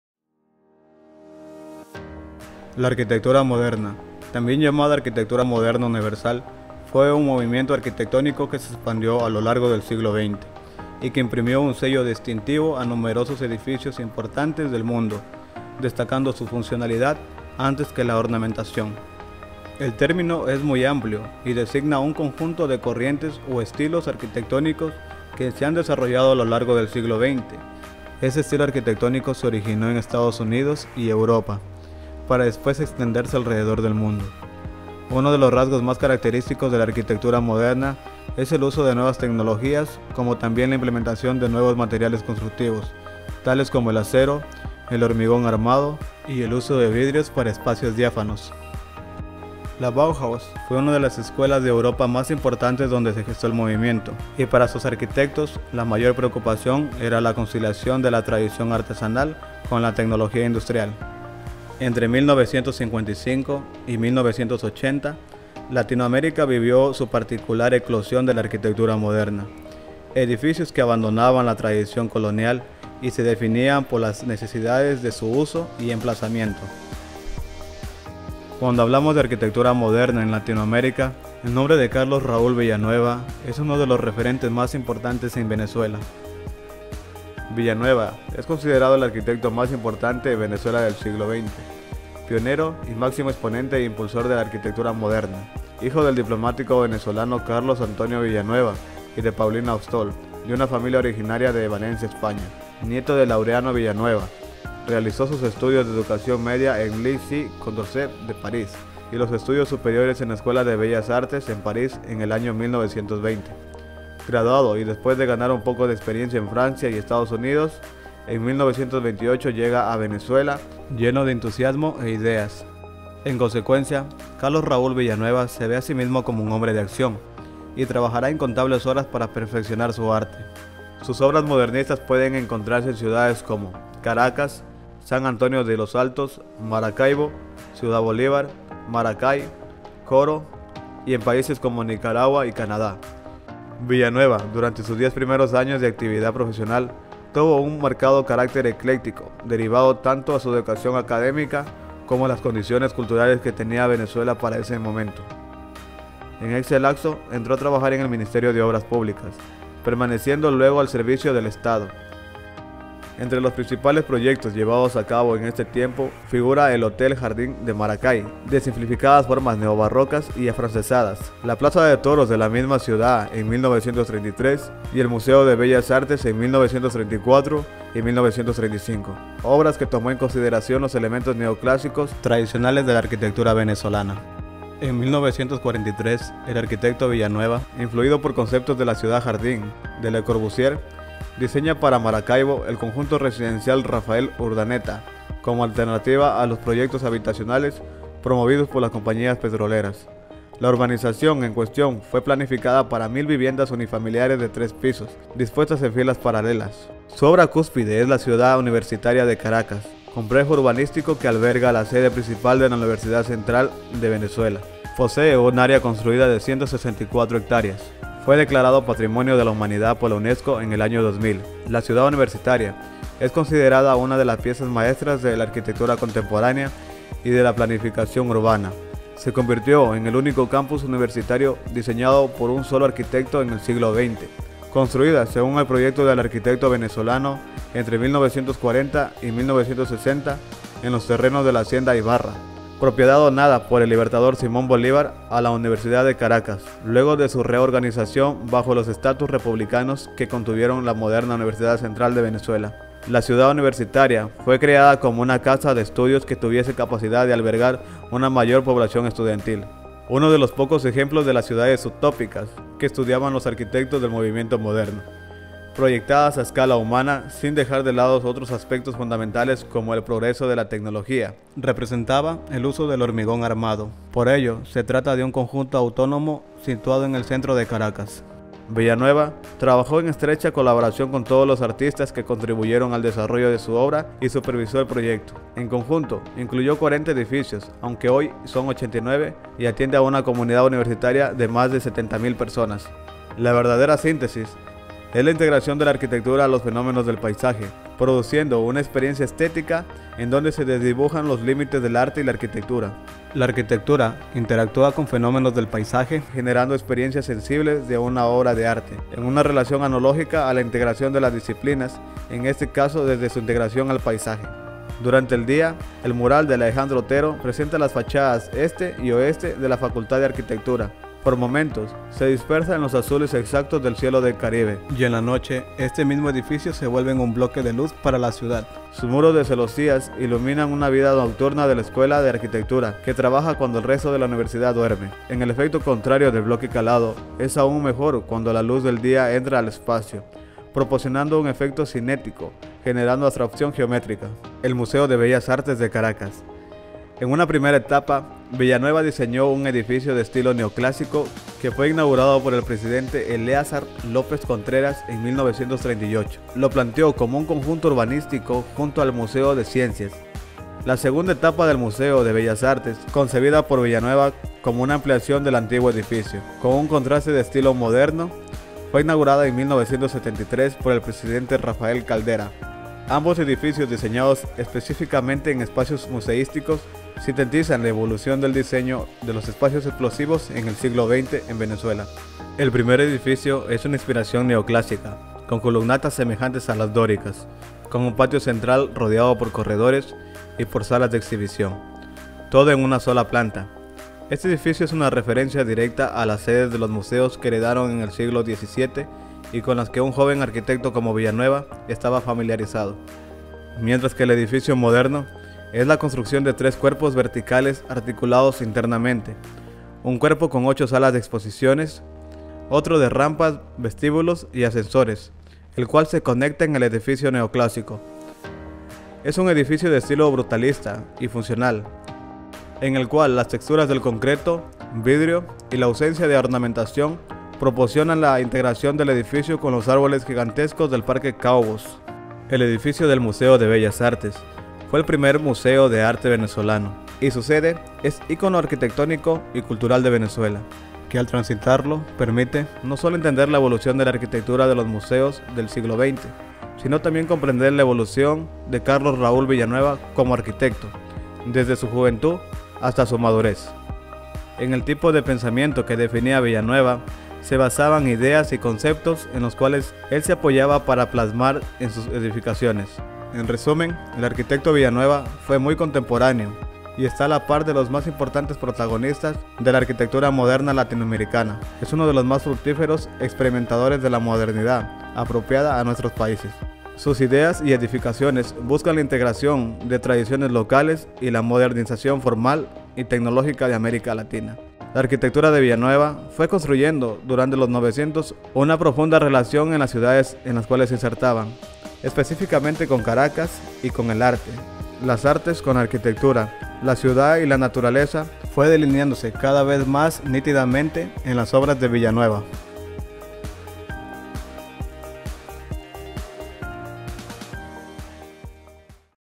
Biografía de Carlos Raúl Villanueva.
-La arquitectura Moderna
-Biografía y obras relevantes del Arquitecto Carlos Raúl Villanueva.
-Universidad Tecnológica Indo américa "Trabajo de sexto nivel de Arquitectura"
-Tutora: Arq. Sonia Cueva
-Elaborador por: Luis Fernando Cañar/ Karla Hernandez/ Grace Pullas.

Creditos:
Videos obtenido: